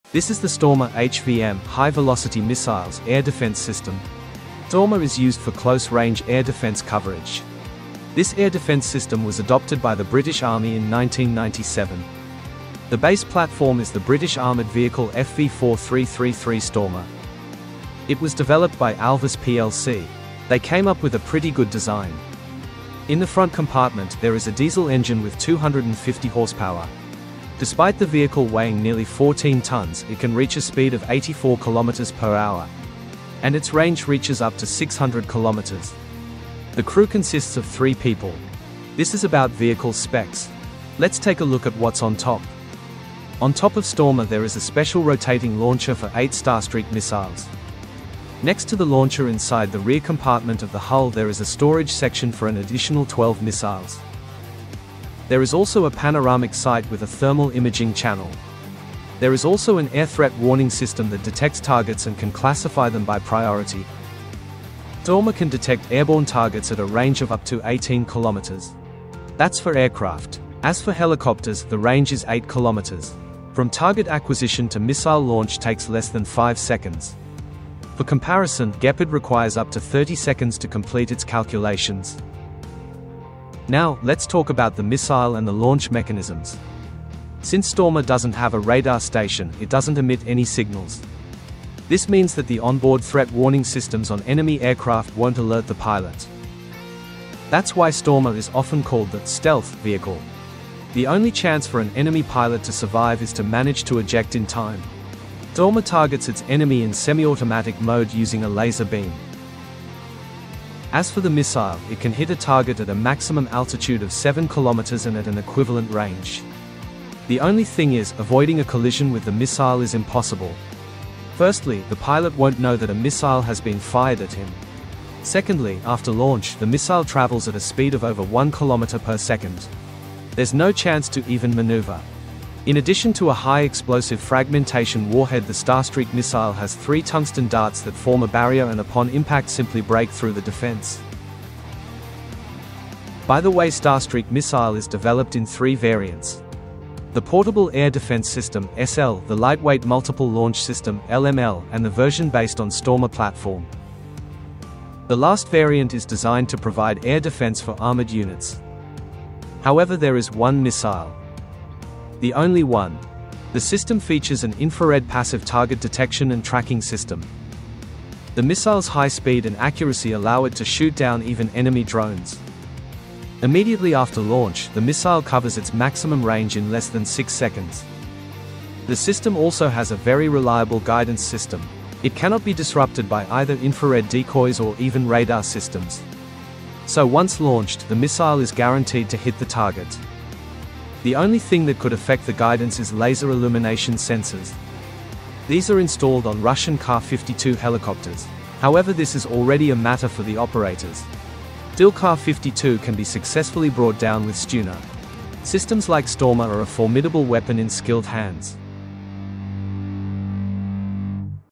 ⚡ FAST. DEADLY. UNSTOPPABLE! The UK’s SHOCKING Anti-Air Power – Stormer HVM
Meet the Stormer HVM – the British-made air defense vehicle designed to hunt down drones, helicopters, and low-flying aircraft with lightning speed. Armed with high-velocity Starstreak missiles, the Stormer delivers unmatched accuracy and mobility on the battlefield. In this video, we explore its features, combat use, and why it has become a crucial asset in modern air defense.